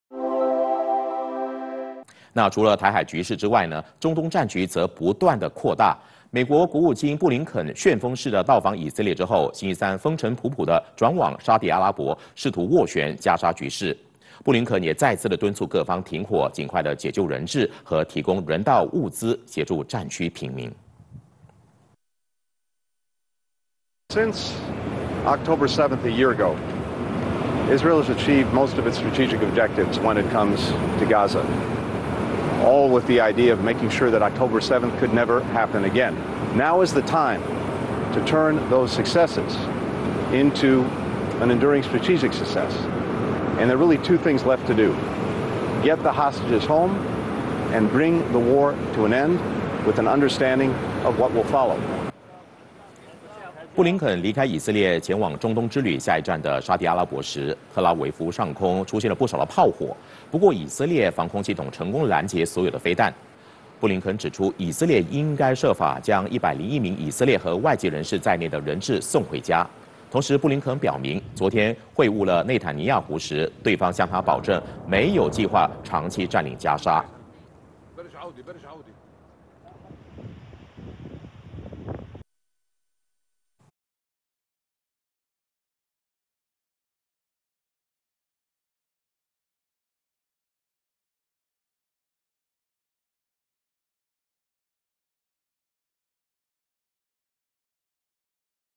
奔波中东斡旋加沙局势 布林肯吁终战解救人质【2024.10.23 八度空间华语新闻】
【奔波中东斡旋加沙局势  布林肯吁终战解救人质】
美国国务卿布林肯敦促以色列结束战火，解救人质。

【今日主播】张傲斌 & 陈嘉荣